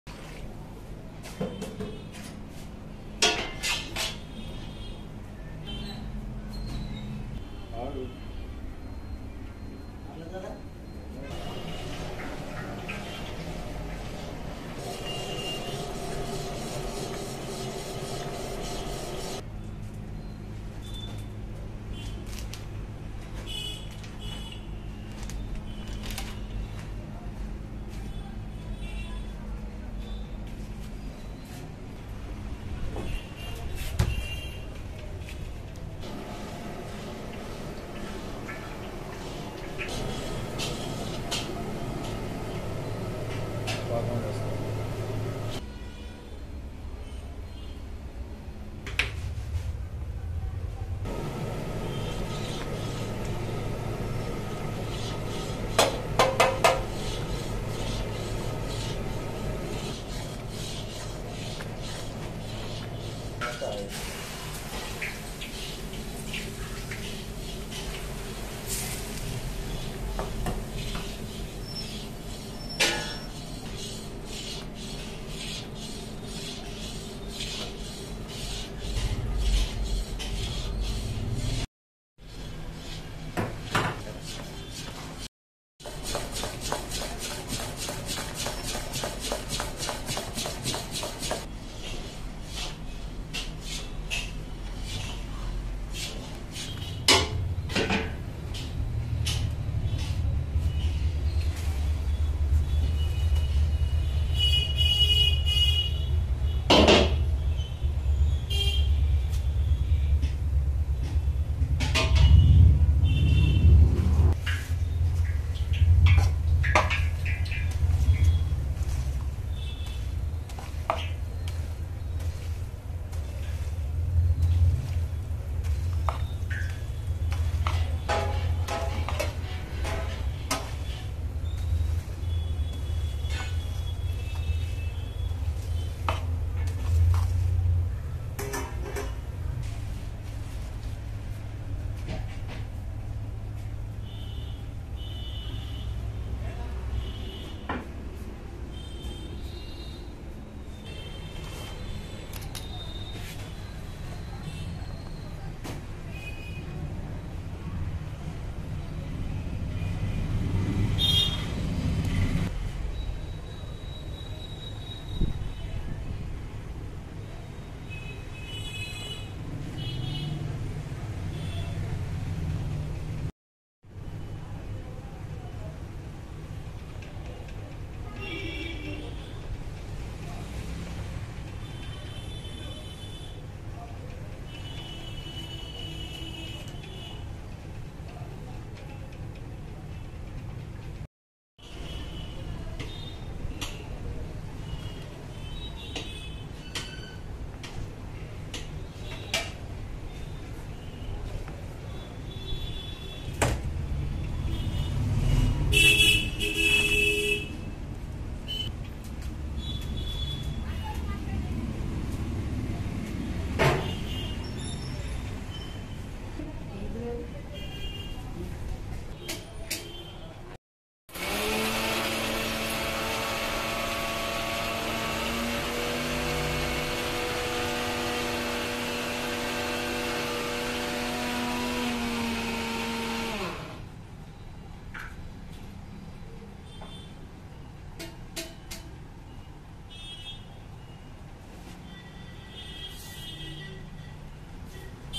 Mysore Pak Making And Cutting - Indian Sweets
He is  Making 7kg of Mysore Pak Sweet  and Cutting into Pieces

ingredients

Ghee or Dalda
sugar
oil
water